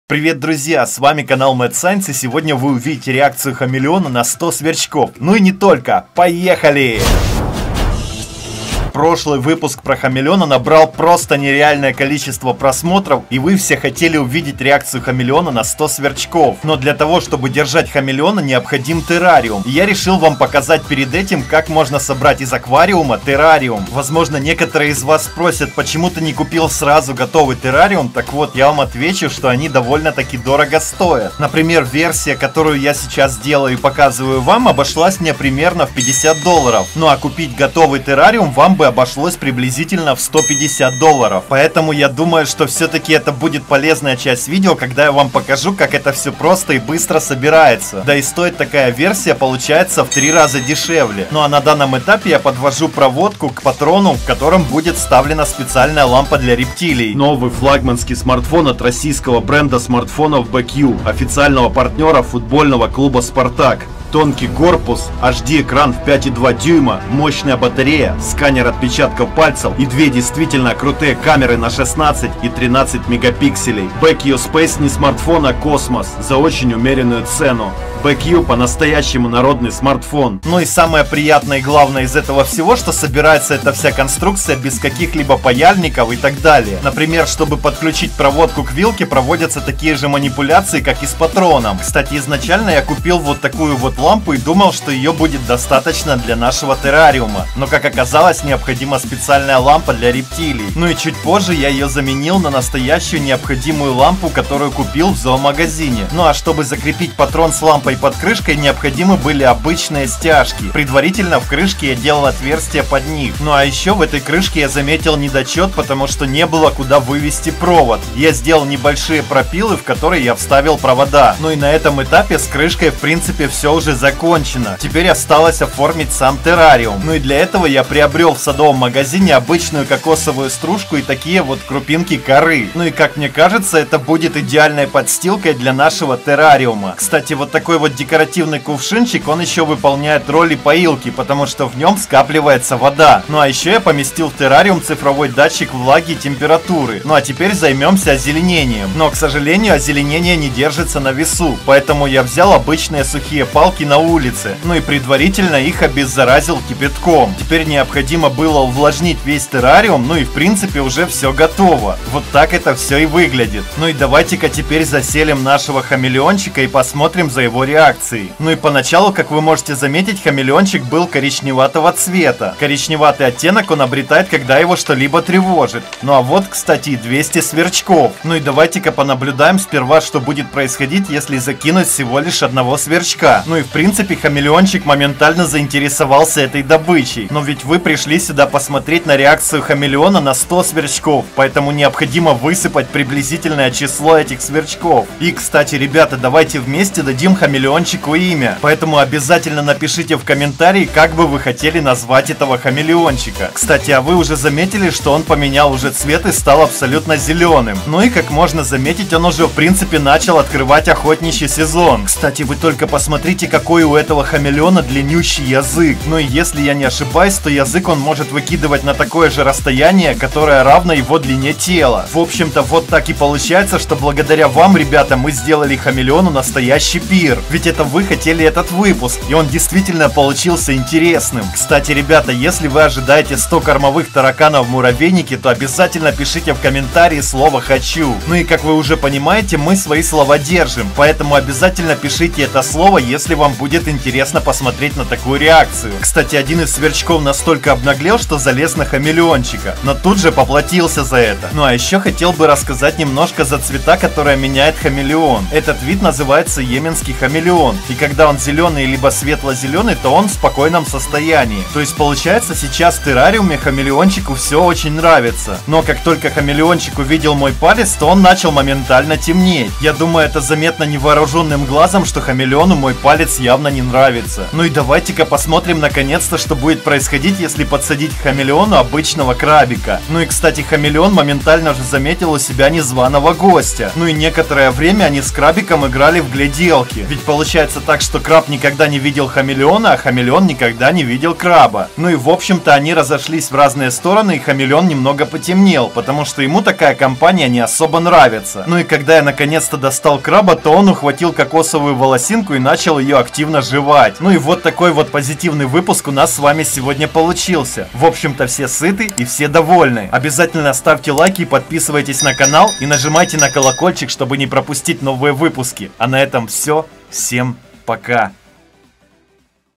ЧТО ЕСЛИ К ХАМЕЛЕОНУ ОПУСТИТЬ 100 СВЕРЧКОВ И КРАБА! ЖЕСТОКАЯ БИТВА - НАПАДЕНИЕ! МЕНЯЕТ ЦВЕТ!
В этом видео вы увидите как можно сделать своими руками террариум для рептилий из аквариума, а если быть точным то как переделать аквариум под террариум, как легко и просто оформить террариум в домашних условиях, а так же вас ожидает эпическая охота хамелеона и 100 сверчков в террариуме! Можно сказать что это битва хамелеон VS 100 сверчков! Такого вы точно не видели! А еще  вы узнаете ответ на вопрос - что будет если к хамелеону опустить большого краба!  А так же что будет если хамелеона посадить на краба! Еще так же узнаете как хамелеон меняет цвет! это очень интересное видео! Надеемся оно вам понравится!